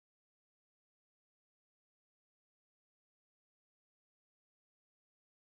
Wait, what happened?
Jacqui rolls carpet over herself while playing